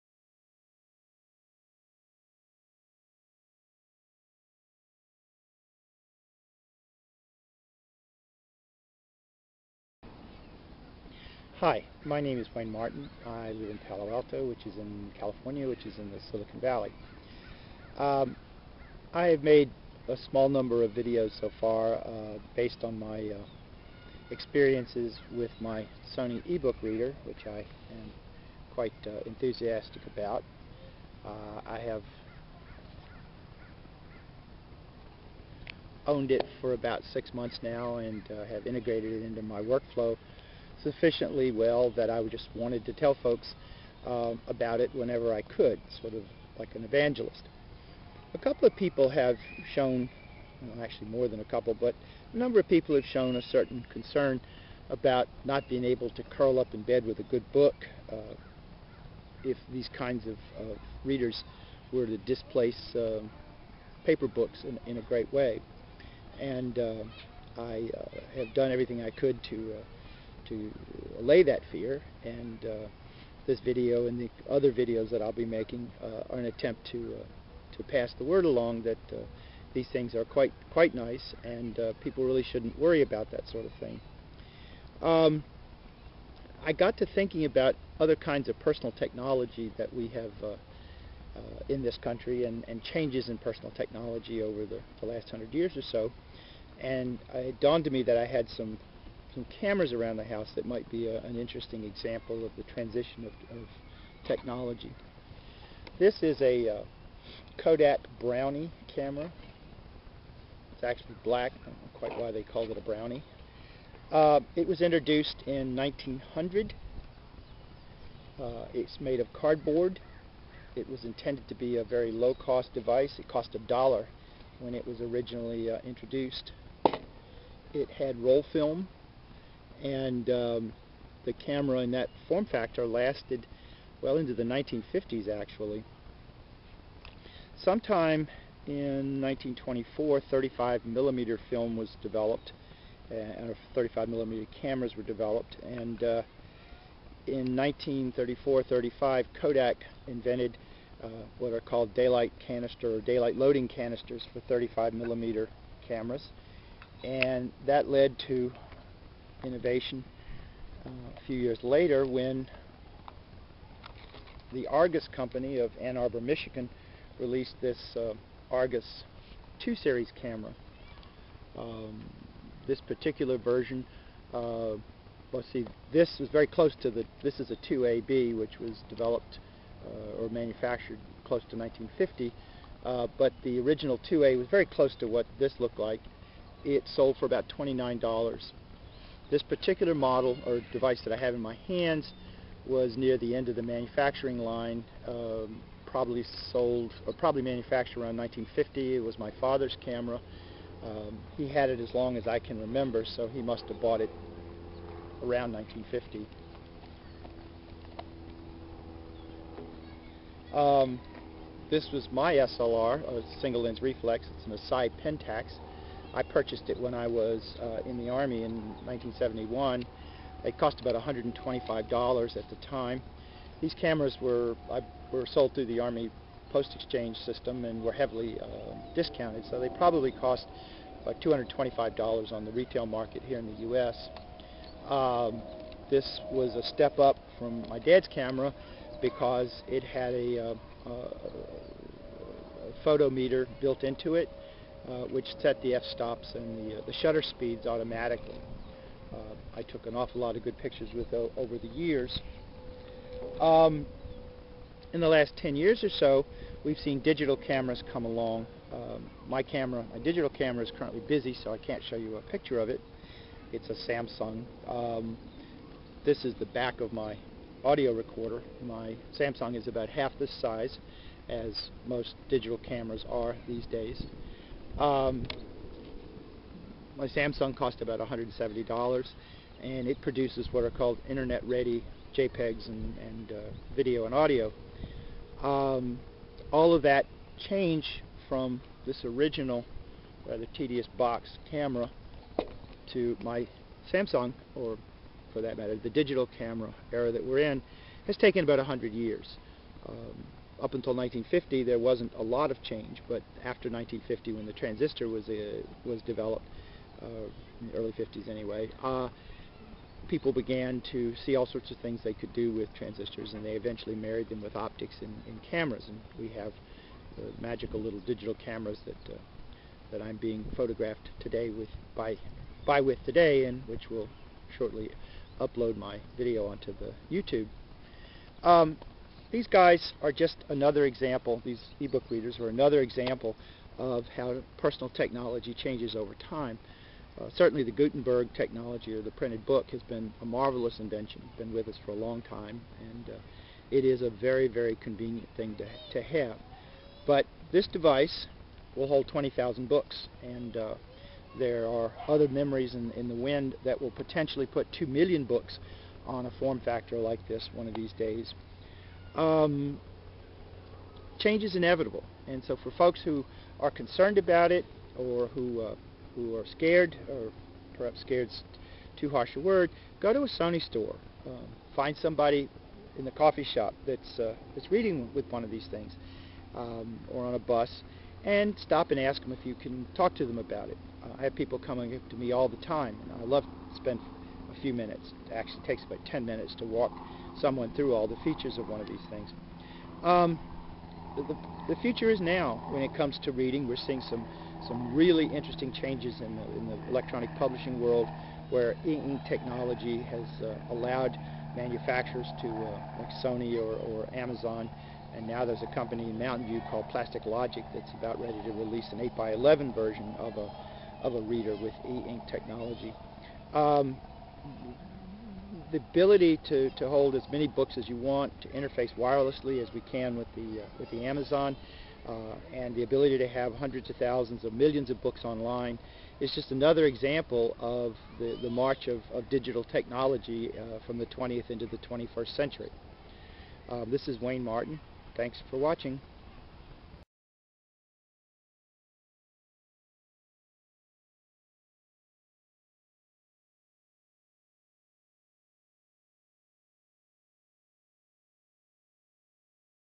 Palo Alto Resident Wayne Martin Parallels Camera Technology To e-Book Readers.
Palo Alto resident Wayne Martin compares the transitions in camera technology to the transitions of paper books to e-books.